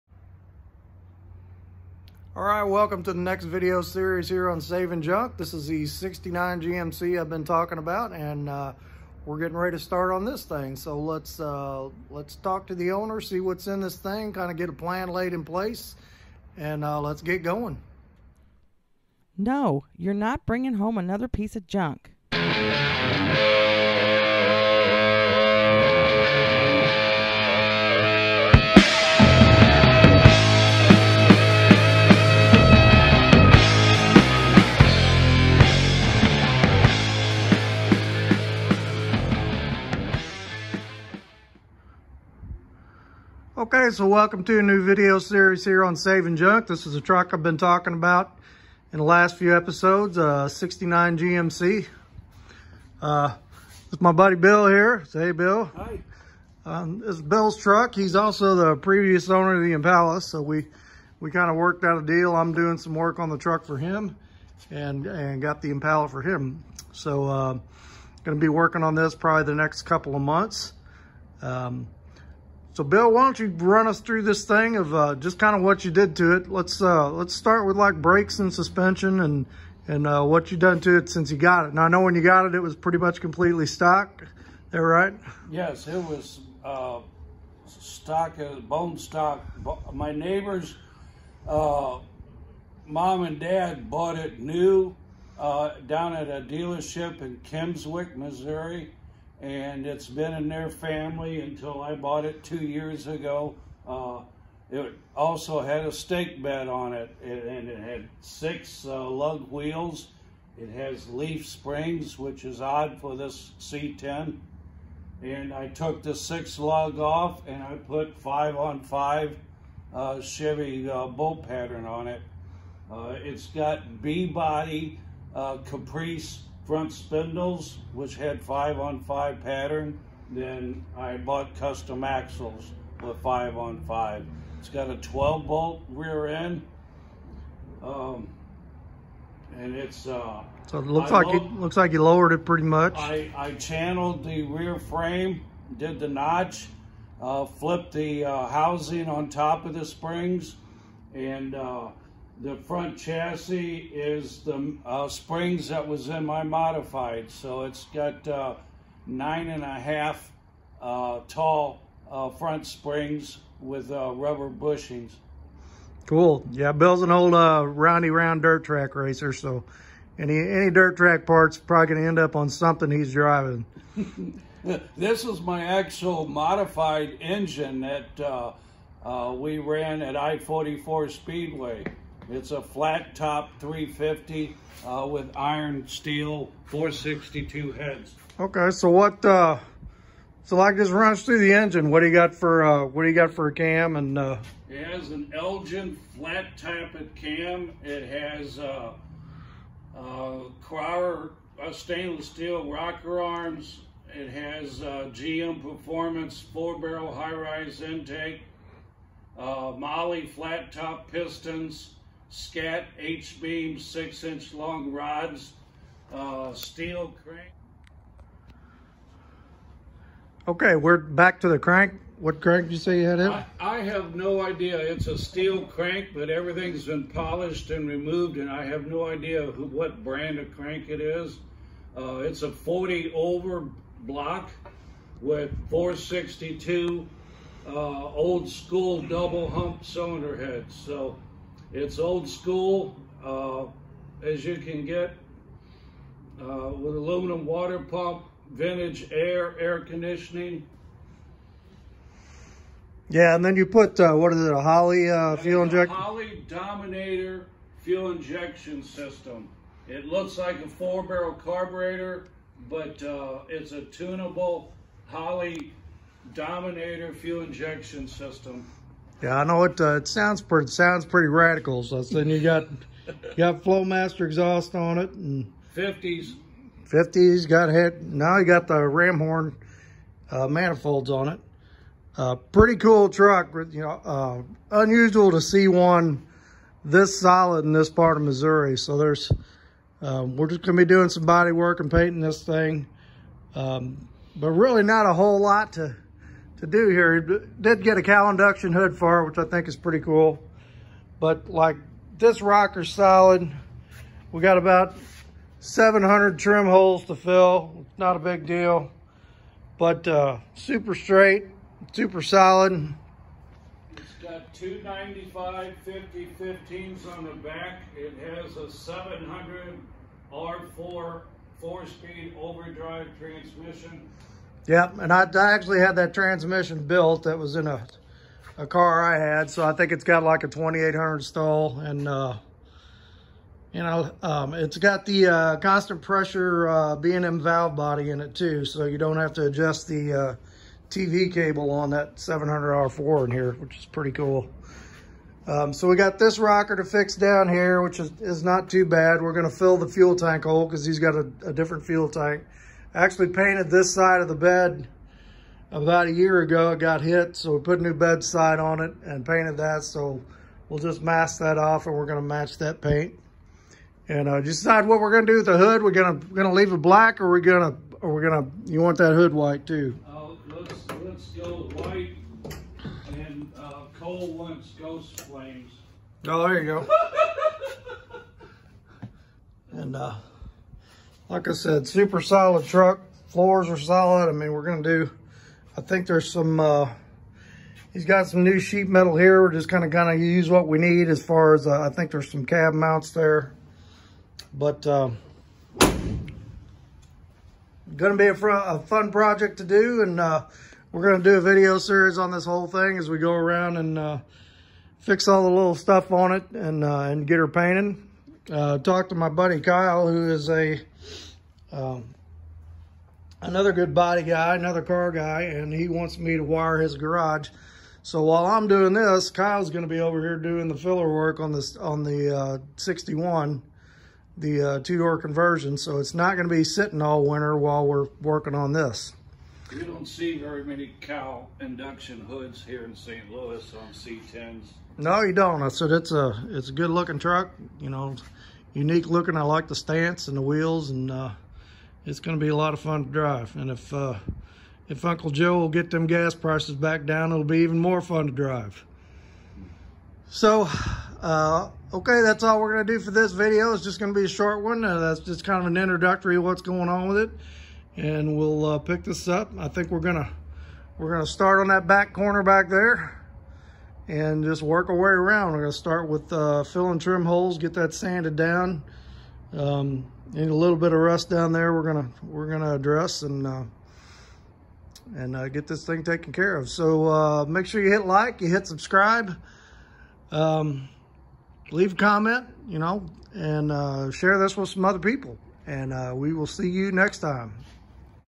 69 GMC Body/Paint part #1
Body work and paint on a cool 69 GMC street truck